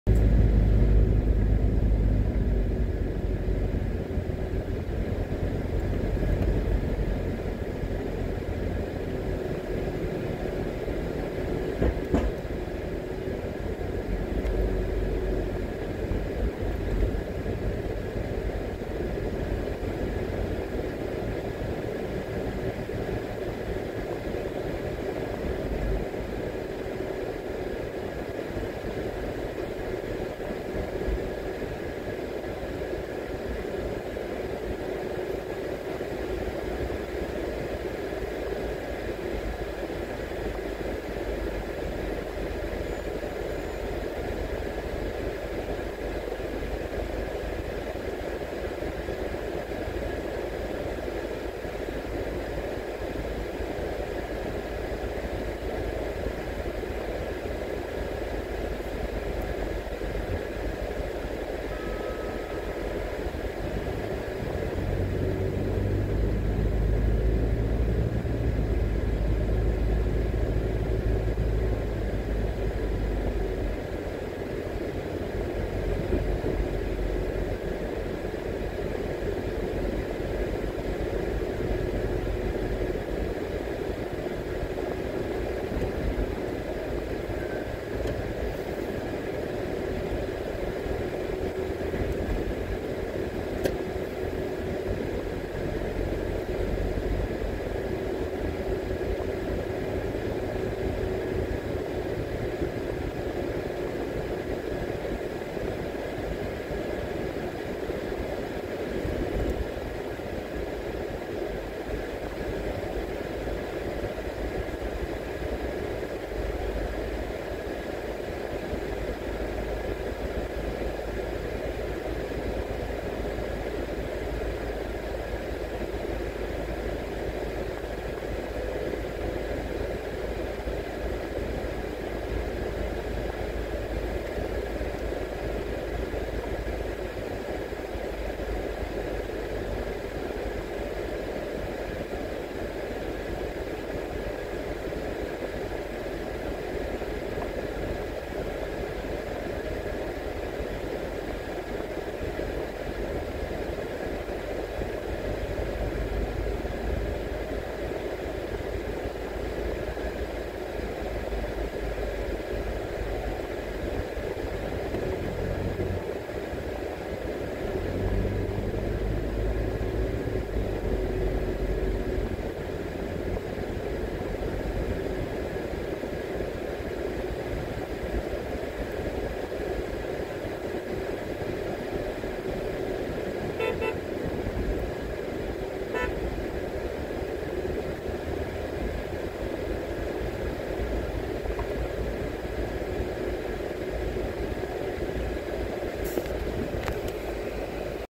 Hagley Park Road | Eastwood Park Road | Jamaica
Driving on Hagley Park Road in Kingston Jamaica.  Driving on Eastwood Park Road in Kingston Jamaica